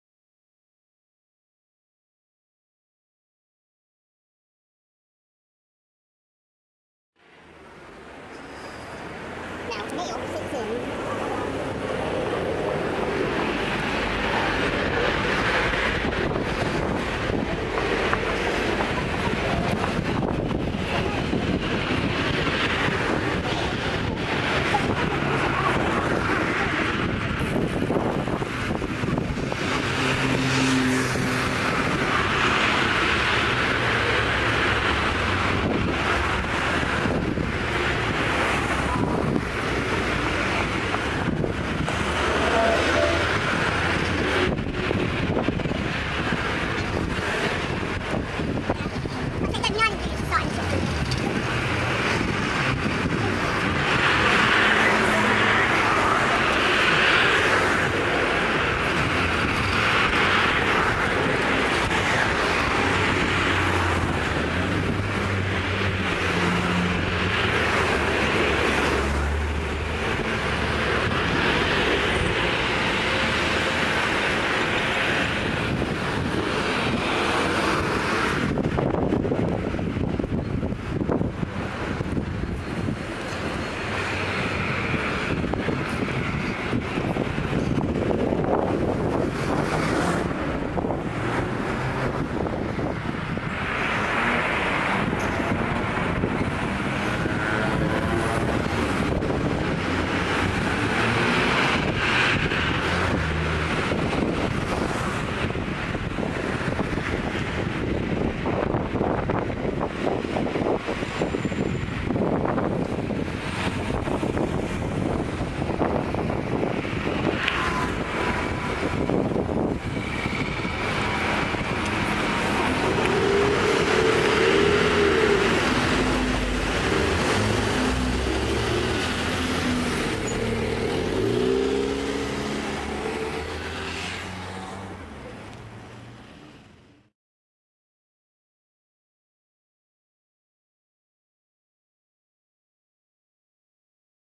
Ashford, Middlesex, (Surrey). Town walk, Part 5 of 6. Feb 2015.
Filmed in sepia and with a little acceleration to give a slightly unreal and retro feel. One of six short films to illustrate a suburban town near Heathrow Airport on a gloomy February day with some drizzle. Lens has some raindrops in this clip!